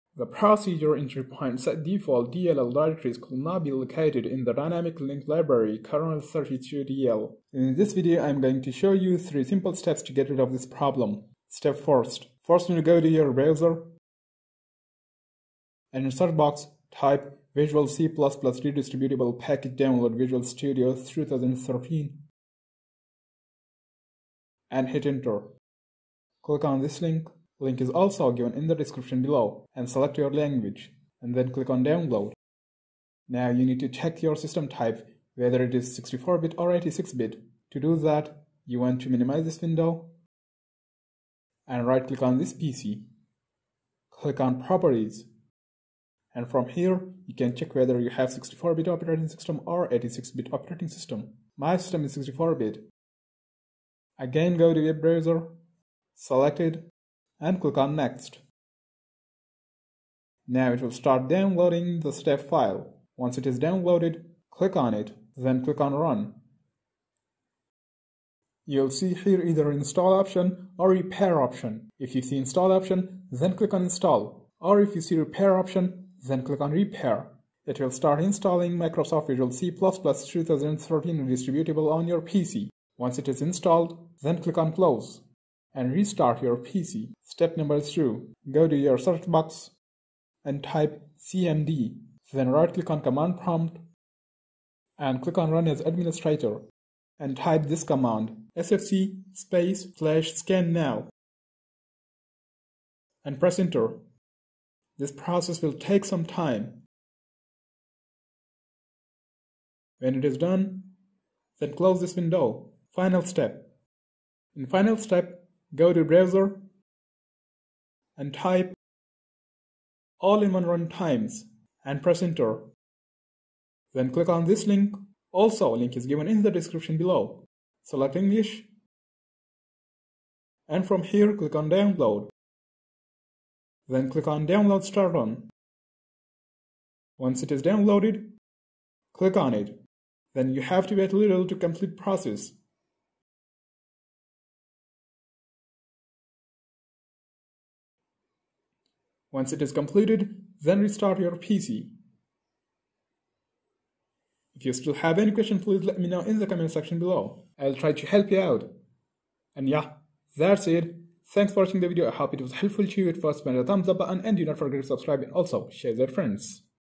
Kernel 32.dll error fixed The Procedure entry point setDefault Directories Could not be Located.
Link for visual c++ redistributable package download visual studio 2013

Link to Video How to bring missing Bluetooth icon to Device manager in Windows 10/8/7:

Hello Friends I hope you doing well,
In this video I have shown you the solution for the problem "the procedure entry point set default dll directories could not be located in the dynamic link library kernel32.dll."
This video contains three simple steps, follow these simple steps and
solved your problem

                                                                                                                      Thanks,